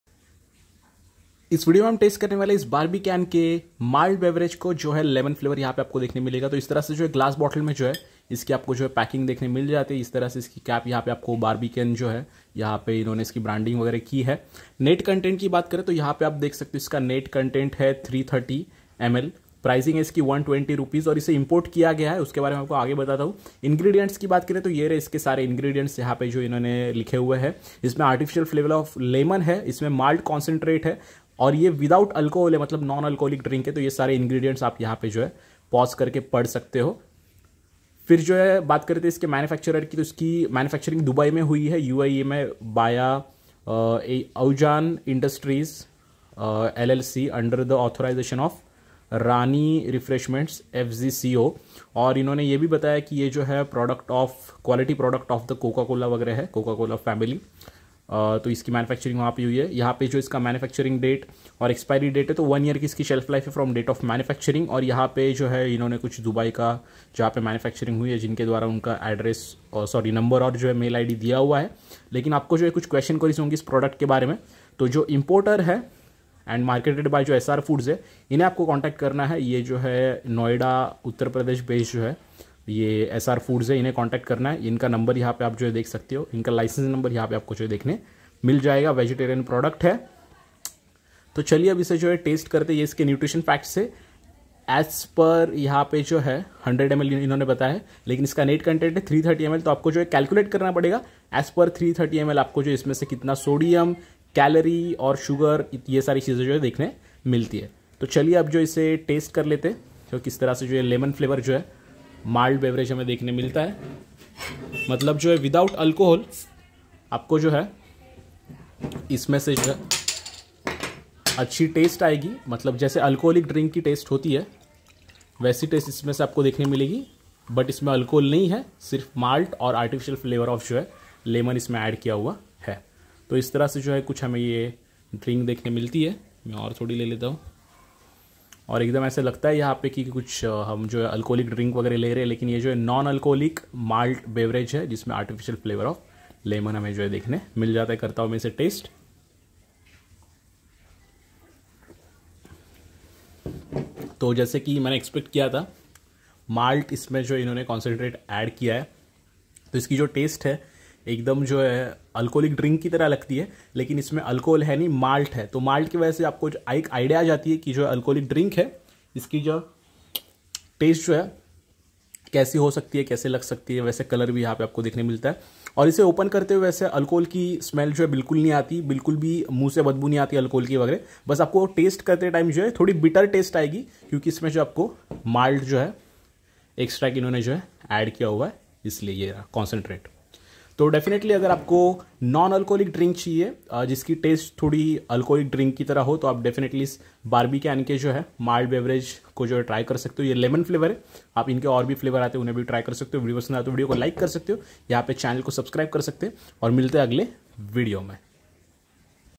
Barbican Malt Beverage Lemon Flavour 330ml | Ingredients, Taste, Price, Ad | Barbican Non Alcoholic🍺
Namaskar Dosto,🙏

Toh iss video mai mene aapko Barbican Non Alcoholic Malt Beer ke bare mai bataya hai.

Barbican Malt Beverage Lemon Flavour - 330ml

Rani Refreshments FZCO
P.O.Box 6845
Dubai, UAE

Dhanywad.!